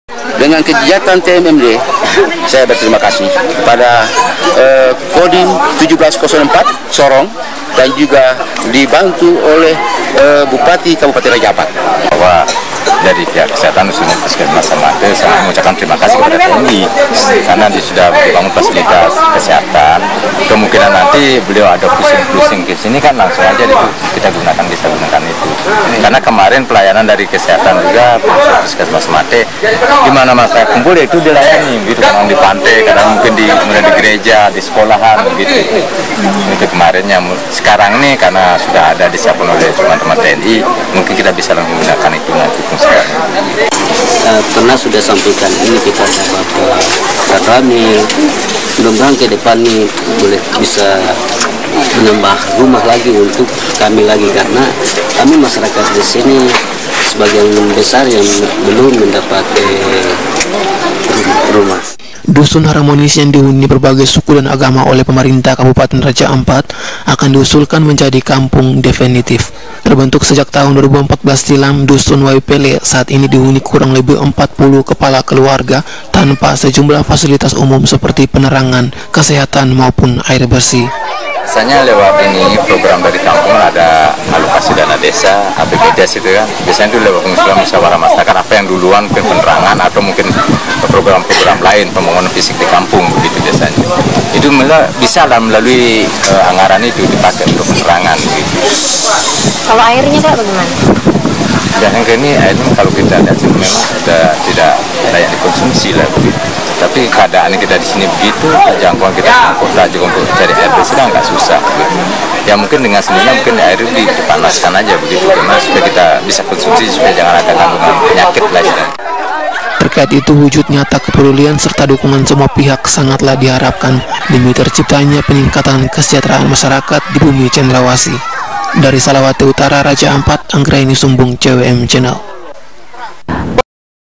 Tokoh Masyarakat ucapan terimakasih kepada Kodim 1704 Sorong Papua Barat dan Bupati
Menolong orang lain akan membuat kehidupan mereka lebih baik, dan begitupun dengan kehidupan anda, dengan ajaibnya semakin membaik. Dan Selama masih ada orang yang ikhlas menolong orang lain, selama itu masih ada harapan untuk dunia yang damai.

TERIMAKASIH